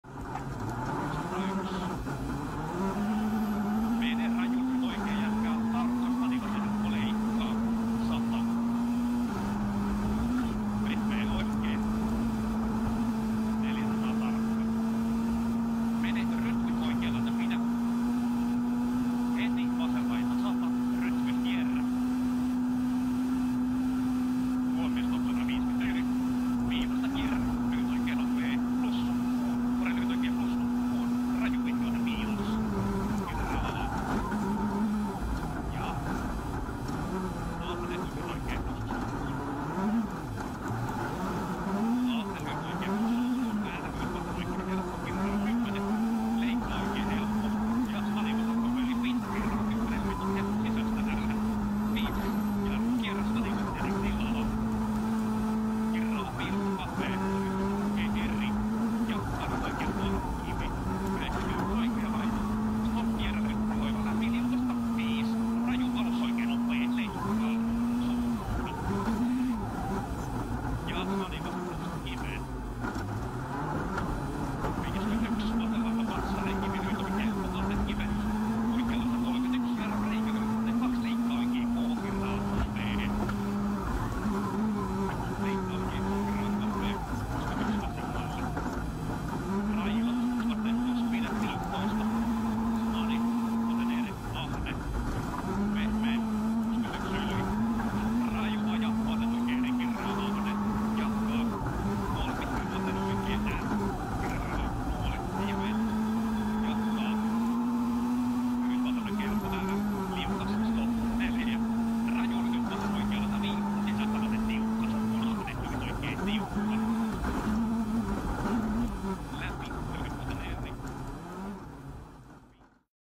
🟥 On board with Rovanperä - Toyota Yaris Rally1 - Rally Kenya WRC 2023
🟩 Rally drivers like to push the limits in racing... and go further. Sometimes it means starting over, other times there are lucky escapes.

🟩 Os pilotos de rally gostam de ultrapassar os limites nas corridas... e ir mais longe. Às vezes significa recomeçar, outras vezes há fugas de sorte.